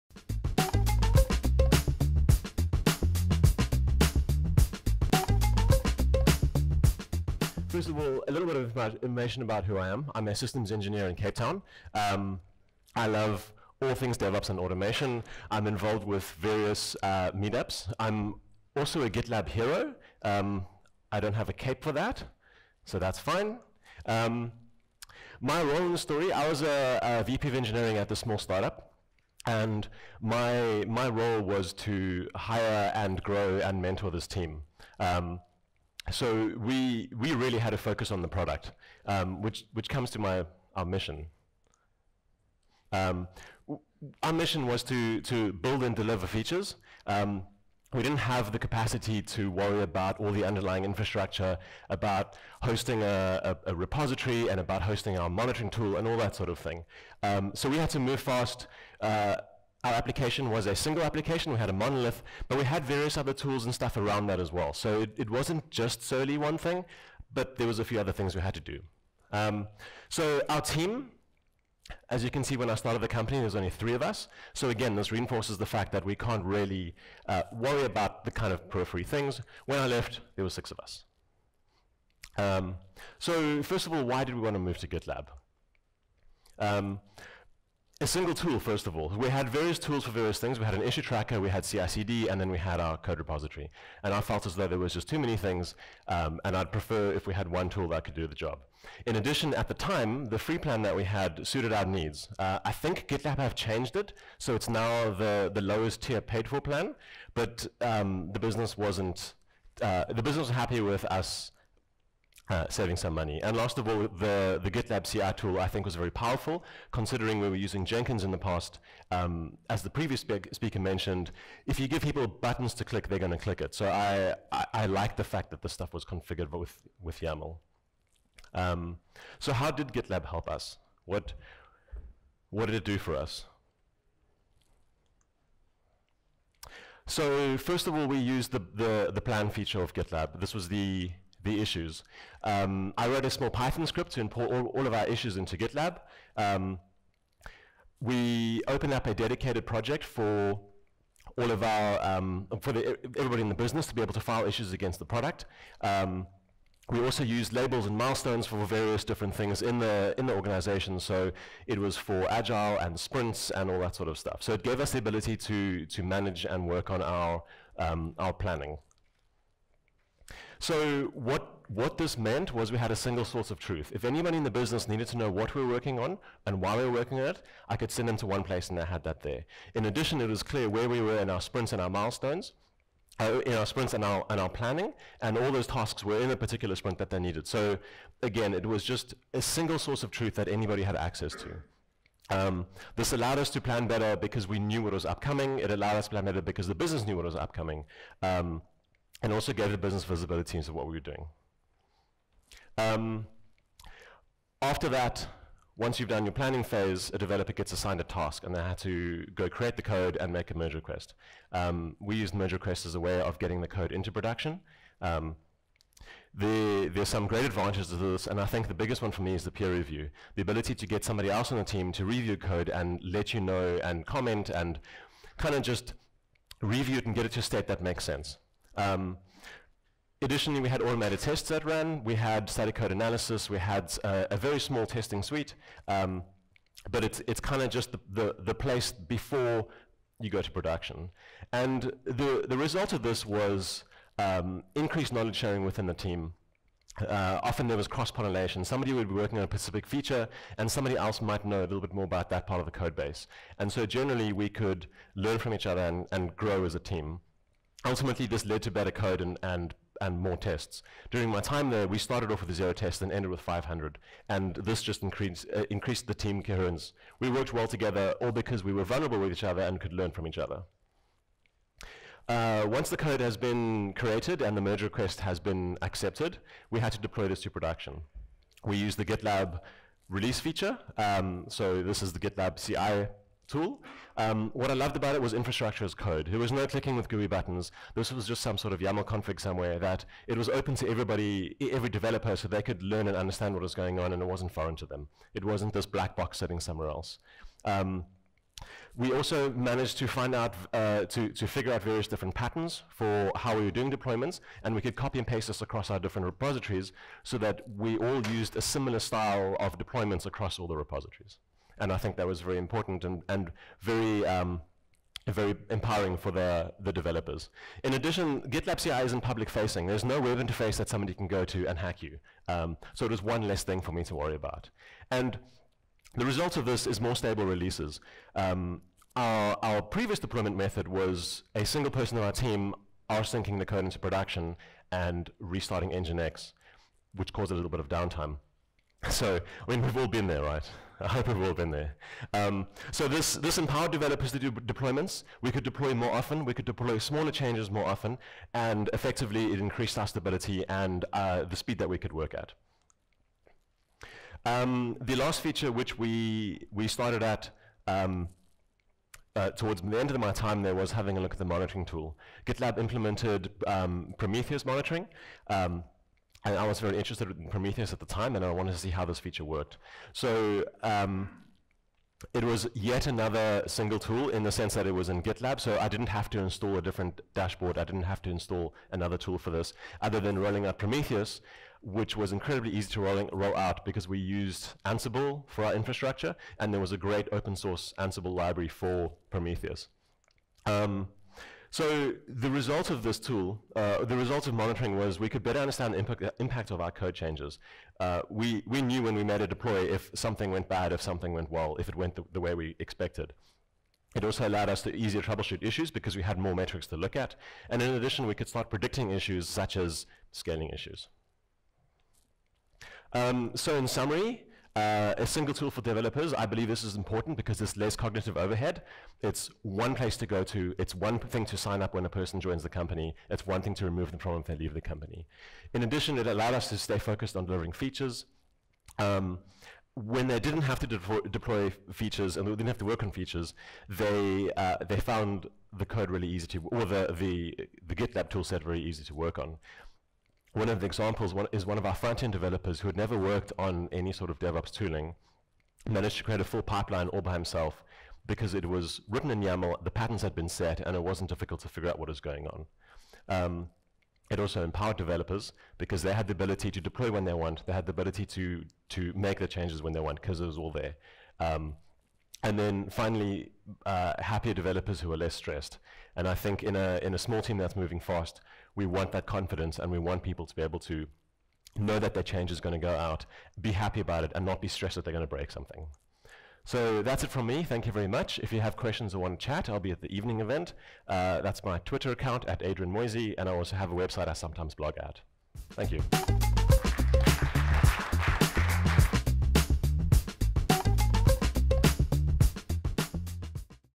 Commit London 2019: How GitLab helped a small team move fast
This is a story about how I used GitLab at a small startup, with only 5 people in the development team. I used GitLab to keep the team focusing on code and building features, and less on the supporting work. In this talk we will look at how the automation of tests and deployments, helped the team move faster and gain confidence. In addition, we look at how the GitLab suite helped with the full product lifecycle (from issue creation to deployment and monitoring).